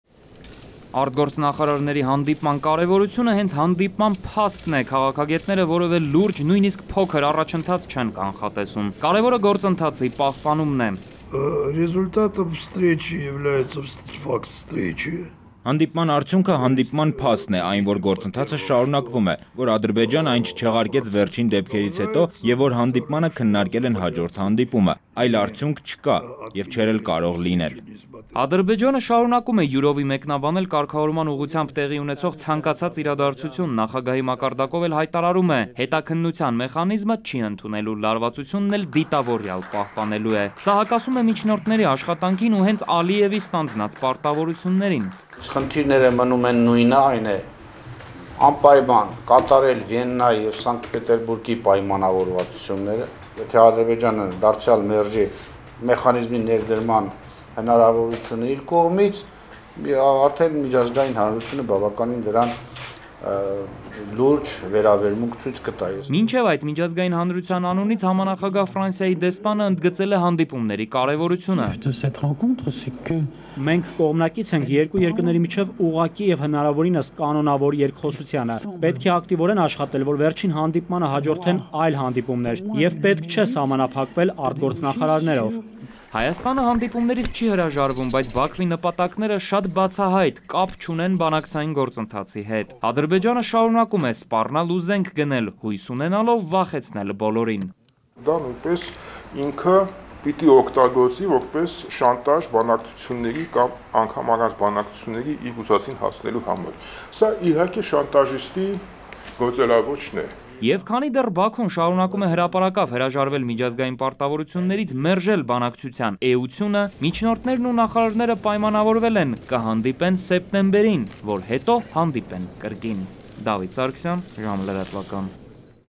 Հայաստանի և Ադրբեջանի ԱԳ նախարարների հանդիպման գլխավոր արդյունքը հանդիպման փաստն է
ԱԳ նախարարների հանդիպման կարևորությունը հենց հանդիպման փաստն է: Քաղաքագետները որևէ լուրջ, նույնիսկ փոքր առաջընթաց չեն կանխատեսում: Կարևորը գործընթացի պահպանումն է: